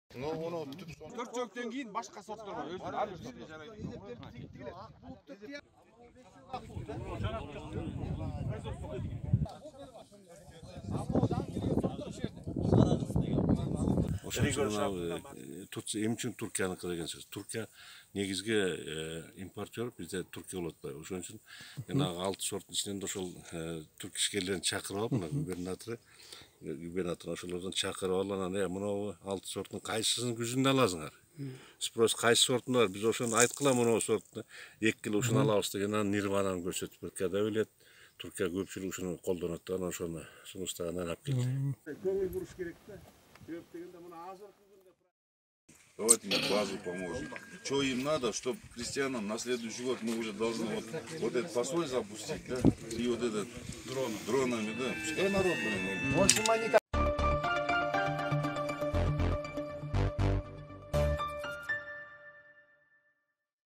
Таласта алгачкы ирээт буурчактын жаңы сорттору сыналууда
Мамлекеттик сортторду сыноо участогунун Бакай-Ата бөлүгү 90 гектар жер аянтын ээлейт. Быйыл анын 31 гектар жерине төө буурчактын түркиялык “Нирвана” сорту эгилген. Аталган аймакка 31-июлда жергиликтүү бийлик башчылары менен бирге дыйкандар топтолук, жаңы сорт менен таанышып чыгышты.